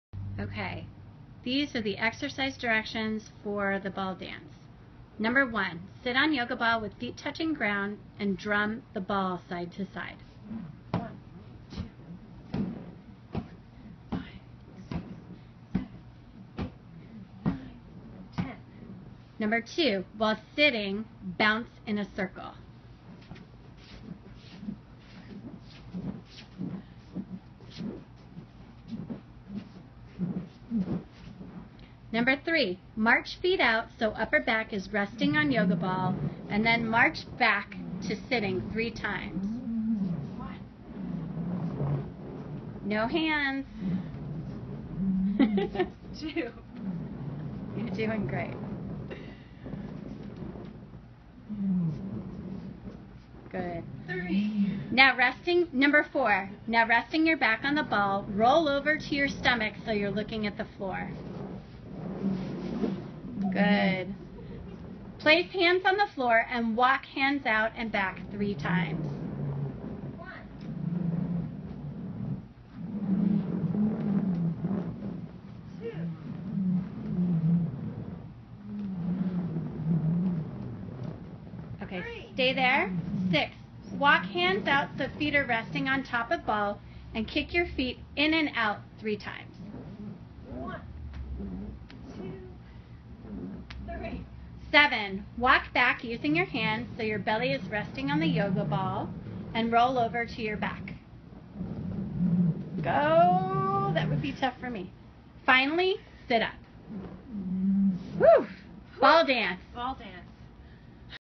Gross Motor Fun- Ball Dance
Here comes the holiday craze! Are you ready... To help harness all that energy as well as work on UE strength, trunk rotation, postural control, and sequencing try the ball dance. To modify practice your favorite parts. See how many rounds you can do!